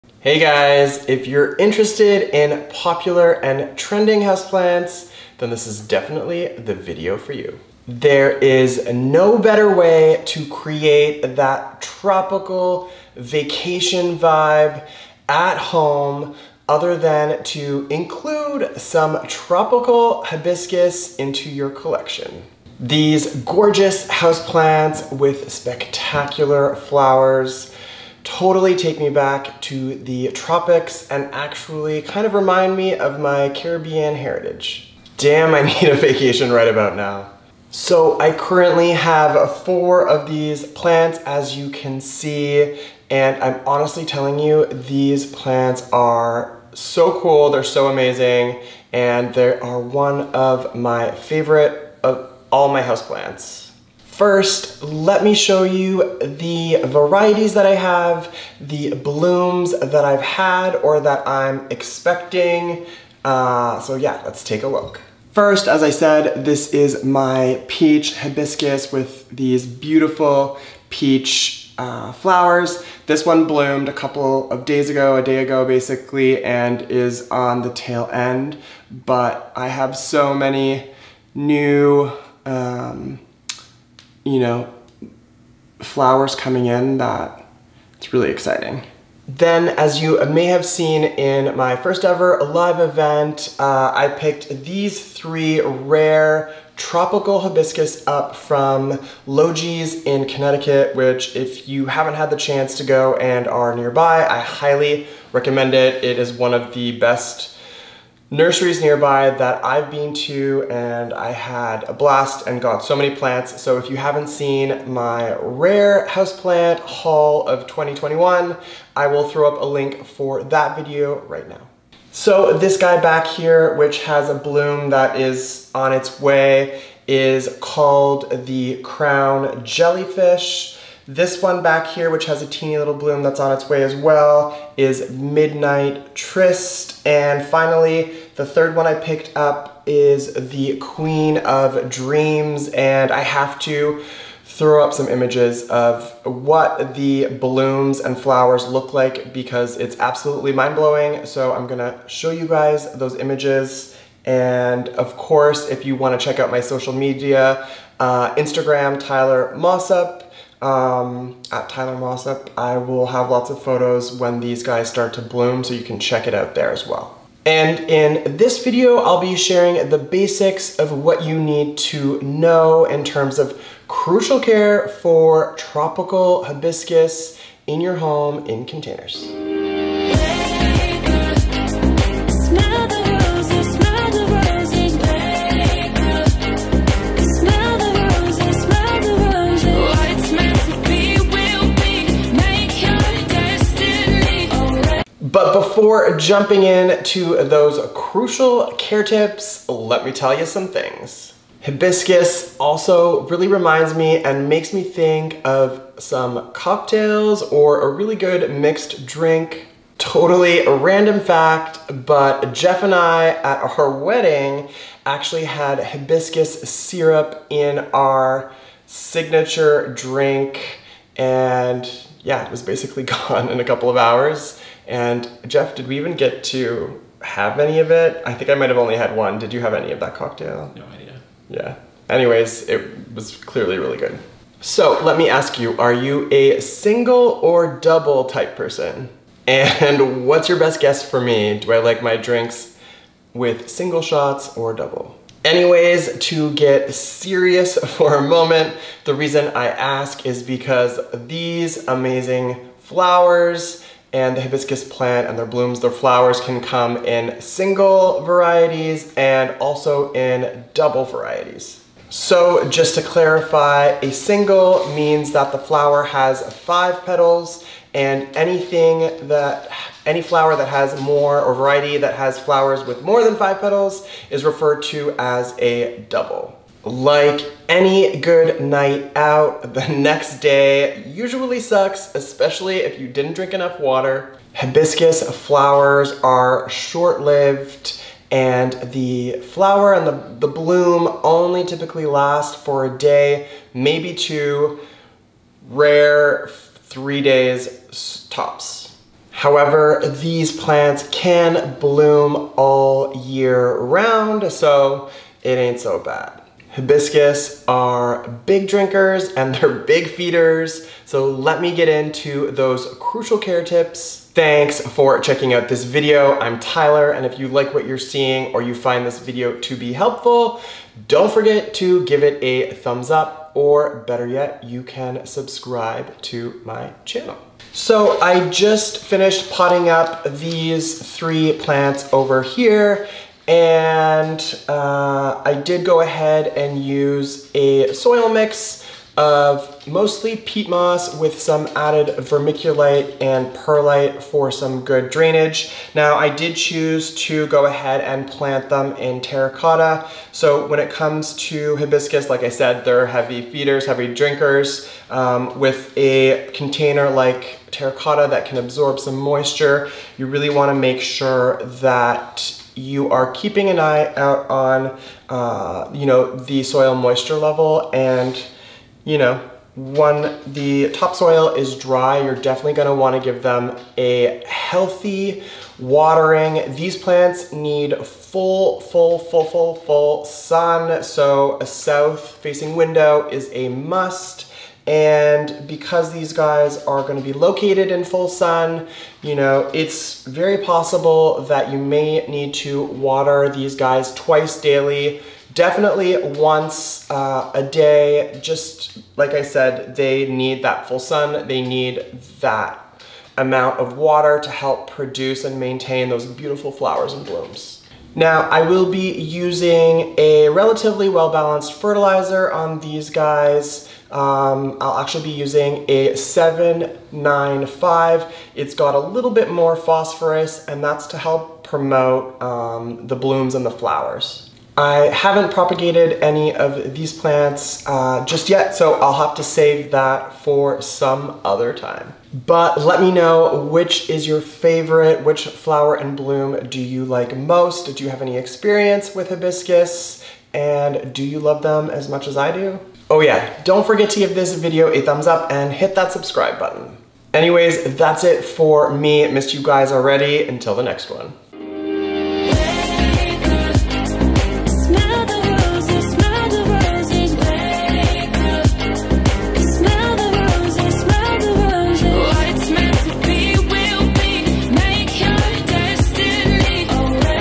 Houseplant Trends 2021: Tropical Hibiscus (From @LogeesPlants) Crucial Care Tips + More!
Tropical Hibiscus is my personal favorite houseplant of 2021! Tyler shares crucial care tips along with some amazing facts about Hibiscus, you can't miss this one.

Tropical Hibisscus:

Plant 1: Crown Jellyfish Hibiscus
Plant 2: Midnight Tryst Hibiscus
Plant 3: Queen of Dreams Hibiscus